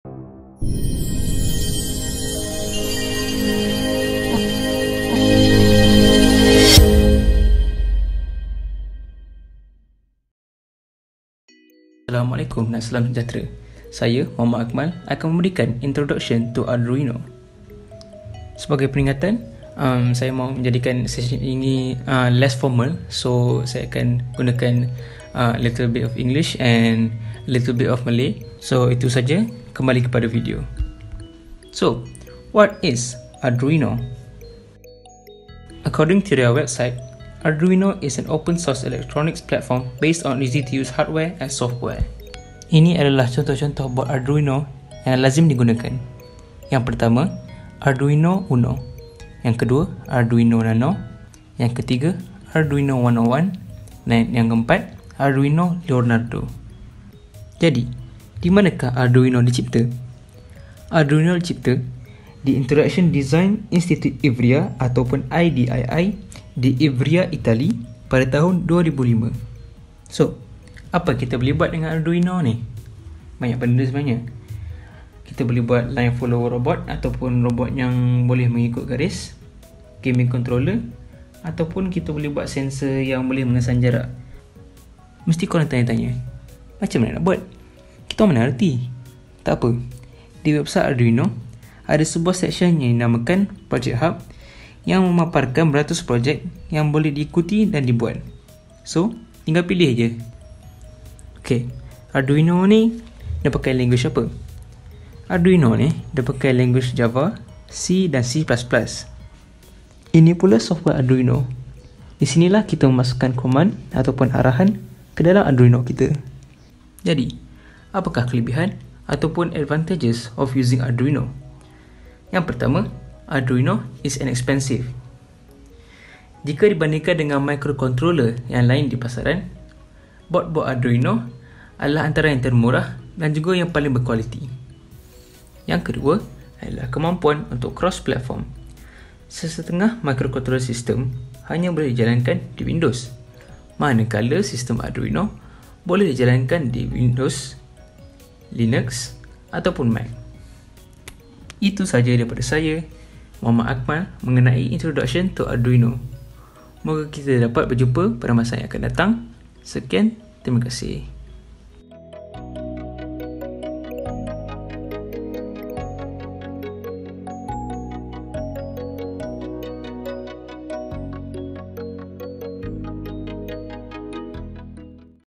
Introduction to Arduino: Part 1 (TKS-PBL)
Interested about Arduino? Want to learn more about it? You have come to the right place!

This is the first part of a two-part series in learning the Arduino. This series is a part of a bigger project known as Project Based Learning (PBL) that will be conducted by Technokeris.